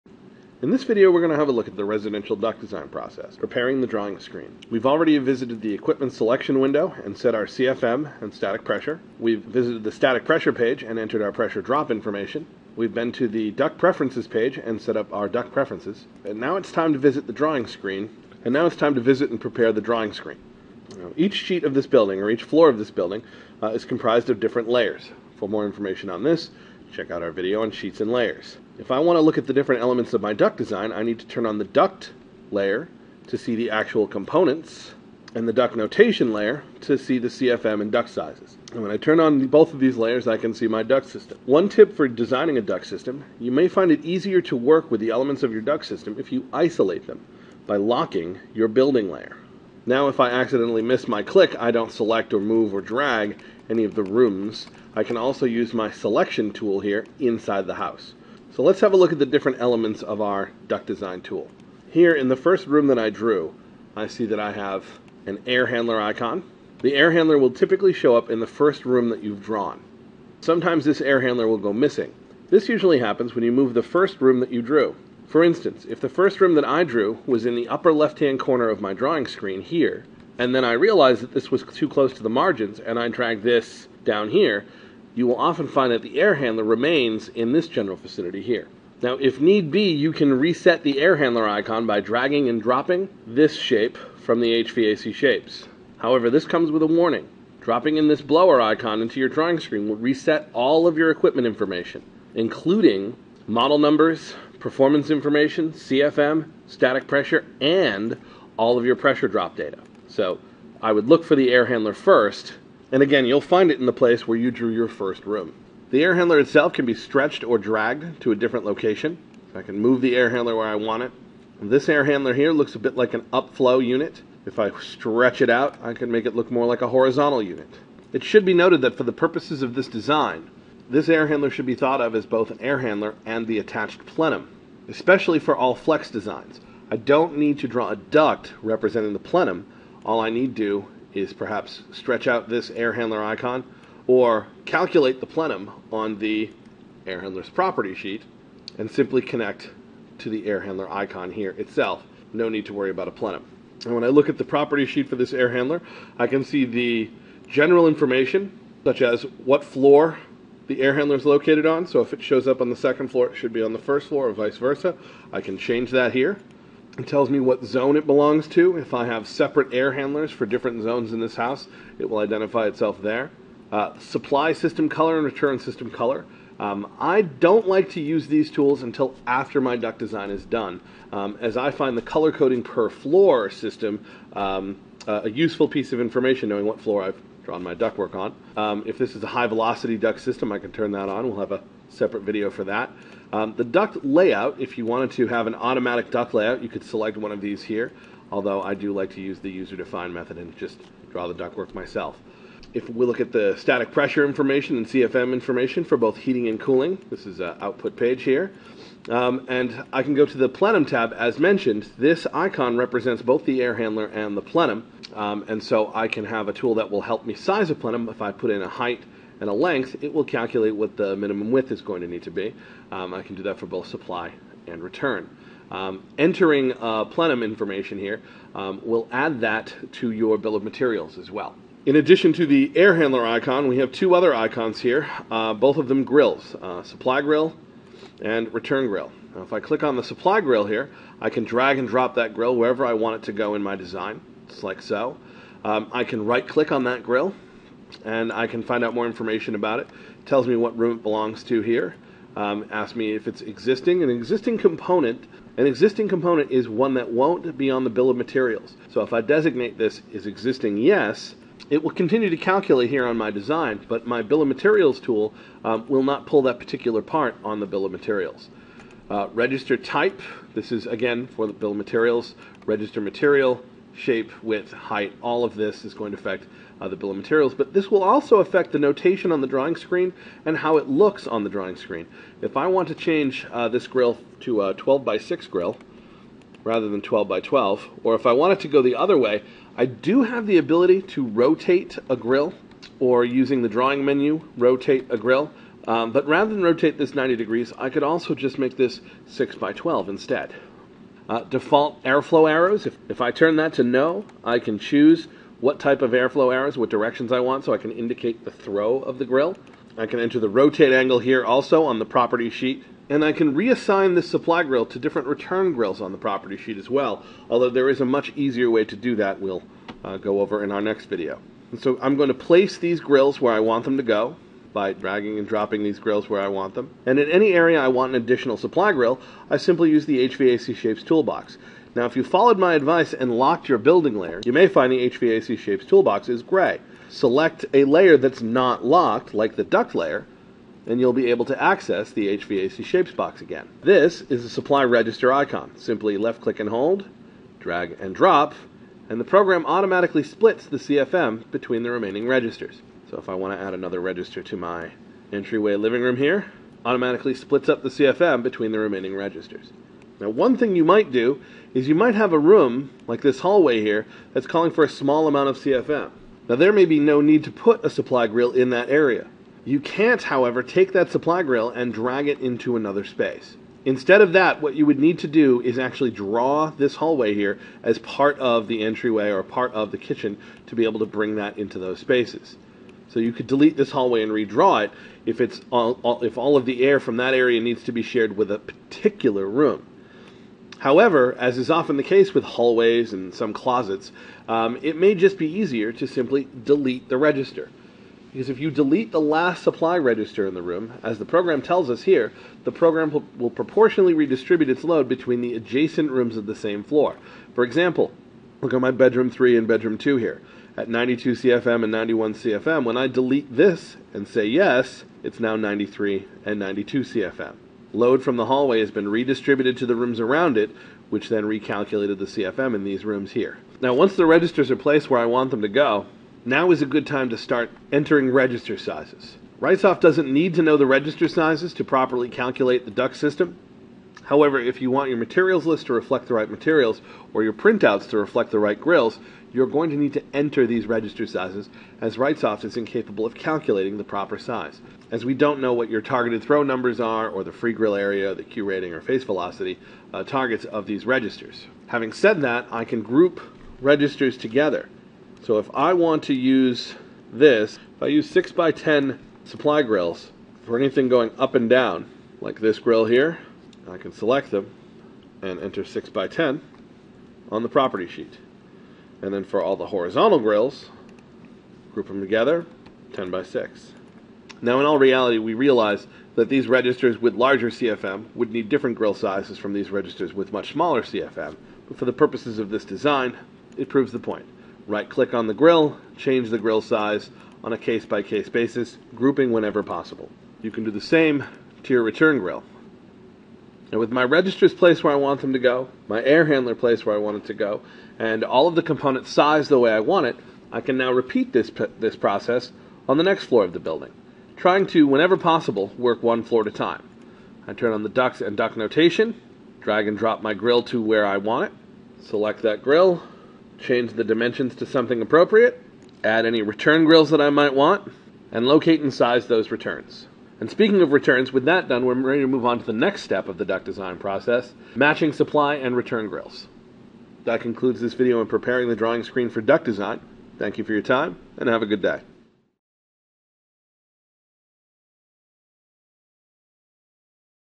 [Wrightsoft] Example Duct Design (Step 4) Preparing the Draw Screen
In Step 4, learn how to prepare the Right-Draw screen to be able to just "drag and drop" design your entire duct system in Right-Suite Universal.

Go through a step-by-step video series of how to design a duct system using Wrightsoft Right-Suite Universal products : Right-D and Right-Draw.